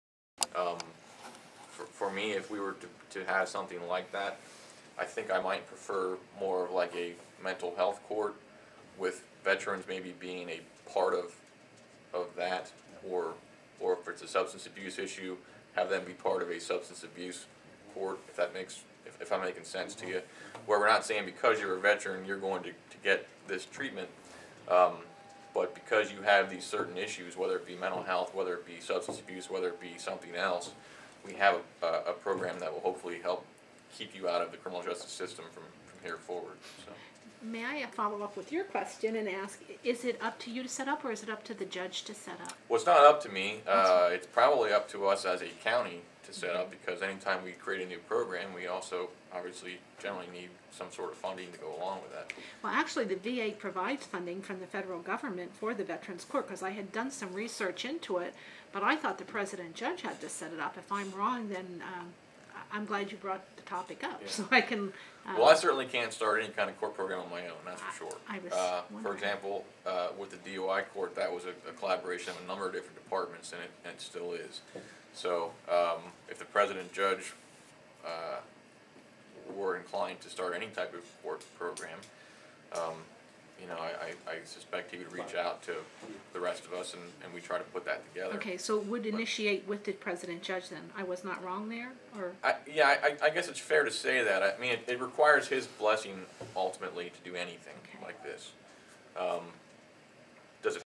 Should there be a Veteran's Court in Lebanon County.wmv
Lebanon PA, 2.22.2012:  District Attorney Dave Arnold says that the President Judge would have to sign off on a Veteran's Court for Lebanon County.  Do you think Lebanon County should have a Veteran's Court?